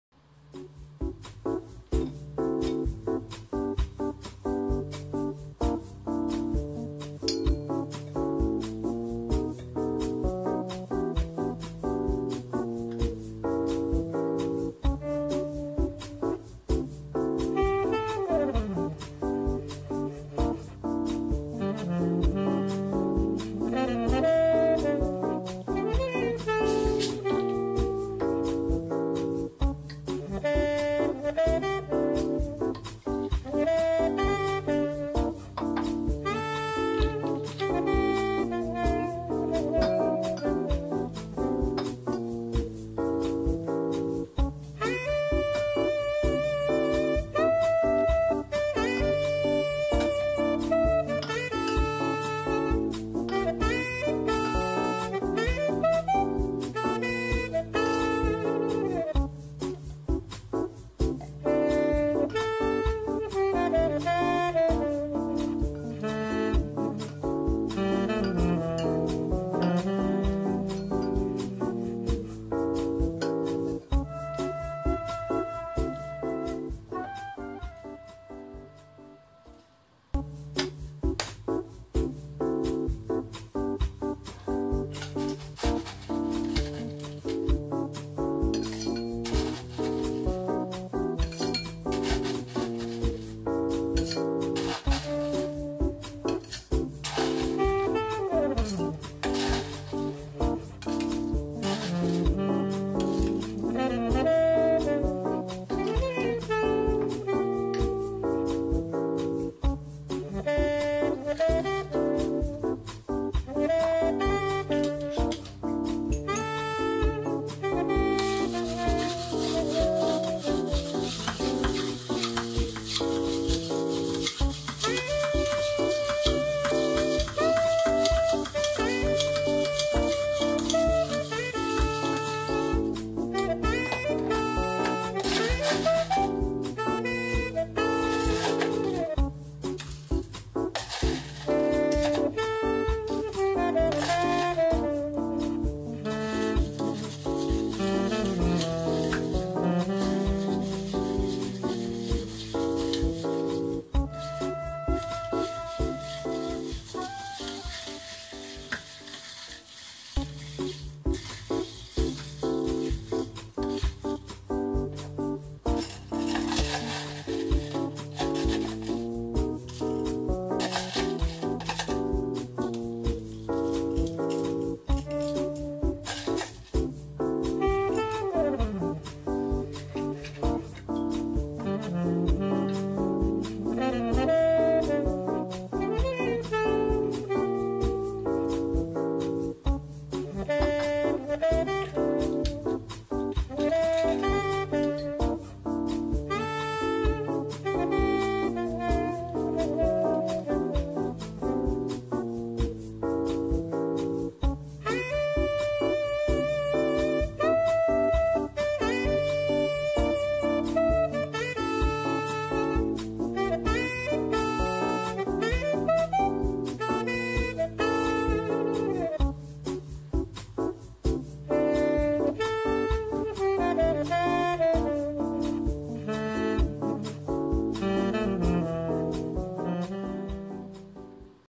カクテル「モヒート」の作り方
カクテル「モヒート」のメイキング動画です。

■材料
ラム(今回はホワイト、ダーク両方使いました！)
ホワイトラム

ダークラム

ライムカット

クラッシュドアイス

今回は最後にダークラムを浮かべることで、
味の重みを少し出たのと、味の違いを楽しめる一杯になりました！
砂糖もシュガーシロップ ではなくサトウキビ由来のブラウンシュガーを使うことでラムとの自然な味の調和があったと感じました。

■こちらのカクテルもいかがでしょうか？

■カクテルブログもやってます！

気になったカクテルは是非作ってみたり、BARで頼んでみてください！
このカクテル動画が、新しい何かと出会うきっかけとなれたら嬉しいです。

＊カクテルの材料のURLリンクはAmazonアソシエイトリンクを利用しています。